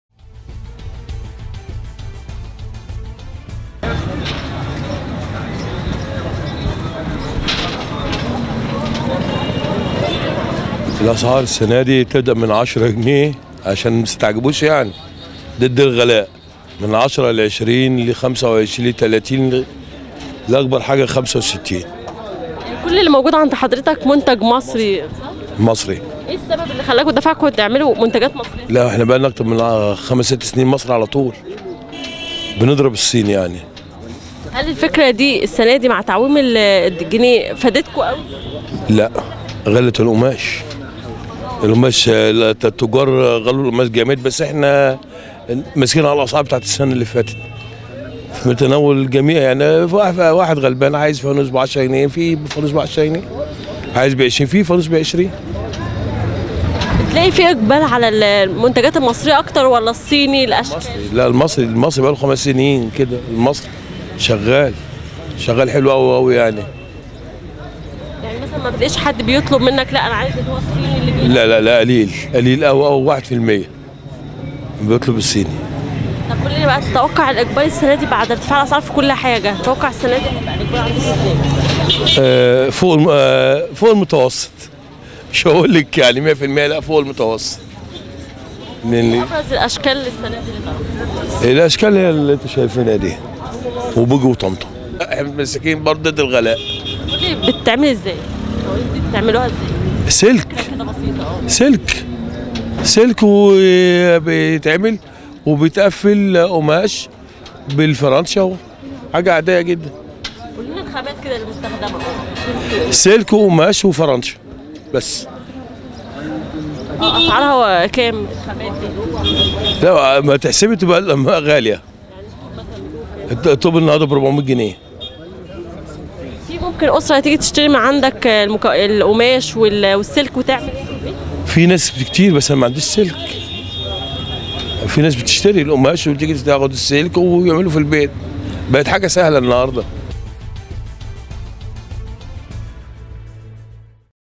مصر العربية | عربية الفول والمسحراتي.. أشكال فوانيس رمضان 2017
يظل فانوس رمضان من أهم الطقوس التى يشتهر بها هذا الشهر والتى تختلف أشكاله من عام لاخر رصدت كاميرا مصر العربية
تصميمات فانوس رمضان لعام 2017 والتى تميزت بالطابع المصرى بعد قرار وقف الاستيراد وكان من اهم الاشكال عربية الفول والمسحراتى .